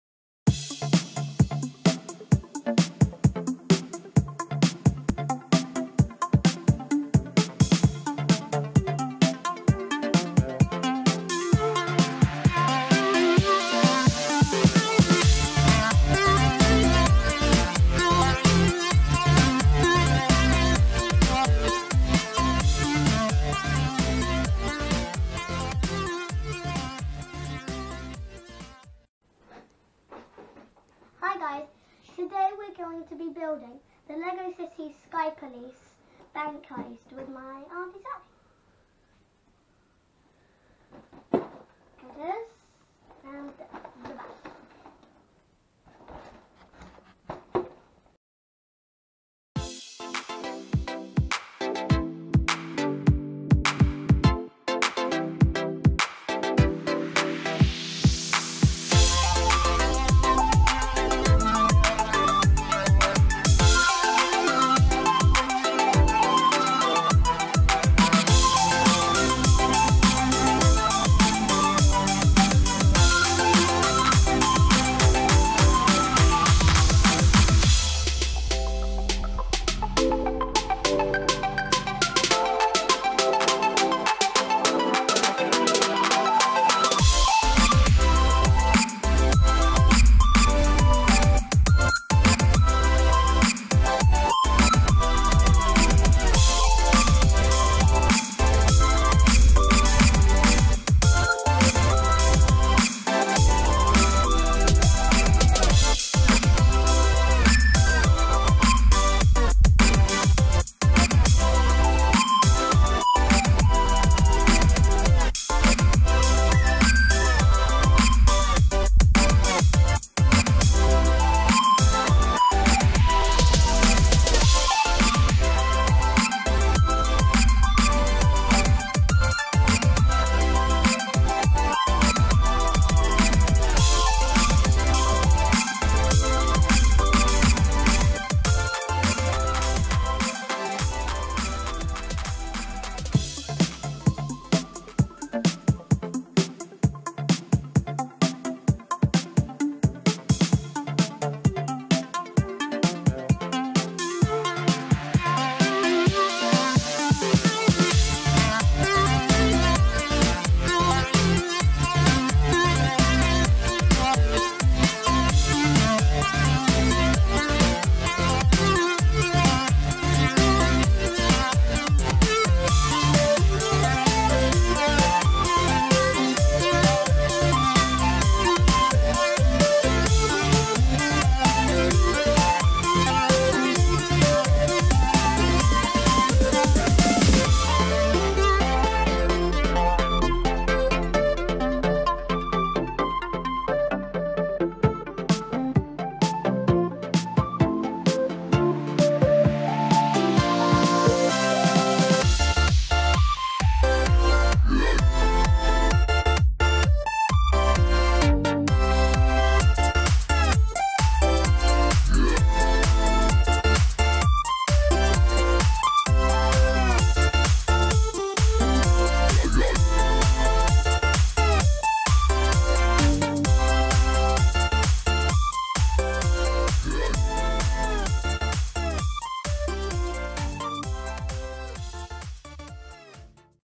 Marleys Toy Madness LEGO City Diamond Heist
Hi everybody.This is my first video for ages, recorded back at Christmas as you'll be able to tell.
I'm re-launching the channel soon more aimed towards gaming but other videos too. Marley